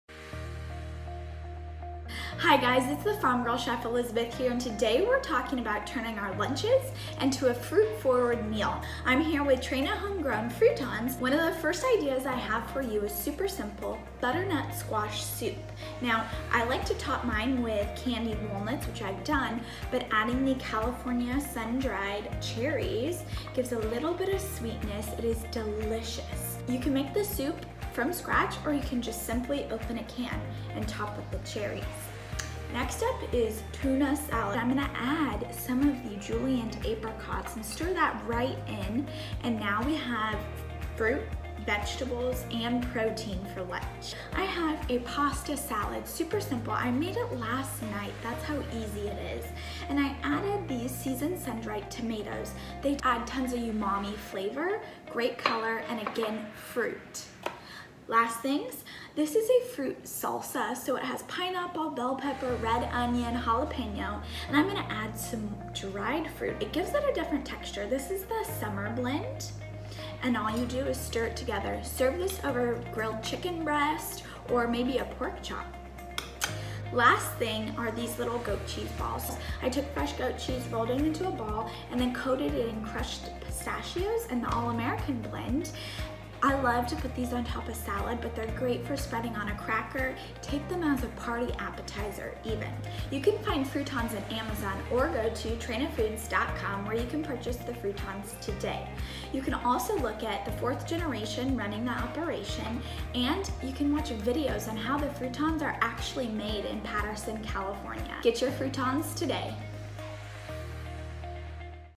Healthy lunches with fruitons
Elisabeth the Farm Girl Chef shares how you can make healthy lunches using Traina® Home Grown fruitons®. From tuna salad to butternut squash soup and more, get ideas for healthy work lunches.